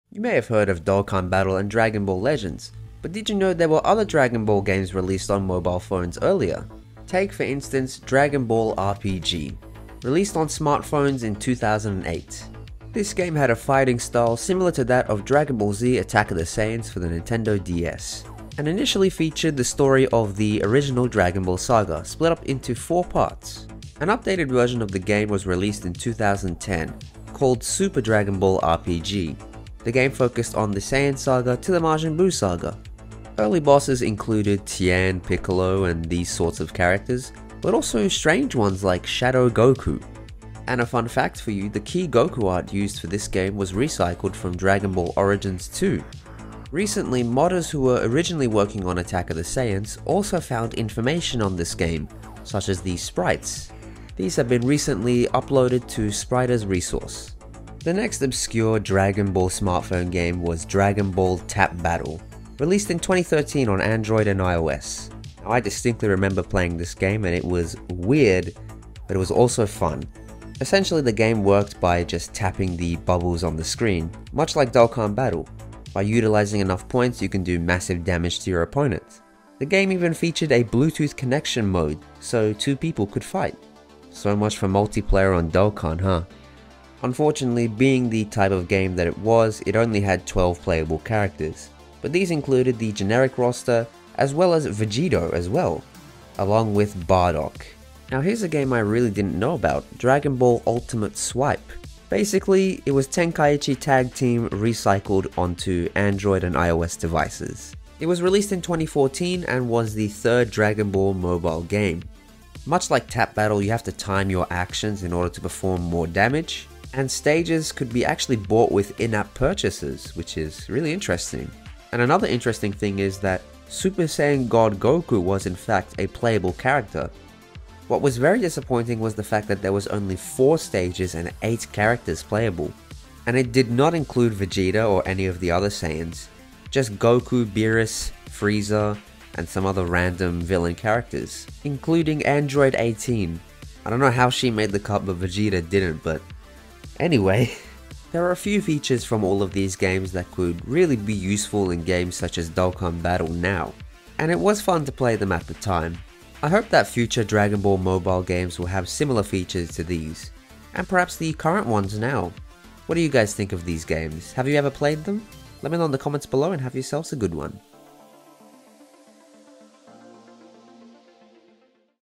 The Lost Legacy of Dragon Ball Mobile Games
The Lost Legacy of Dragon Ball Mobile Games: Before Dokkan and Legends, there were some other DBZ games for smartphones…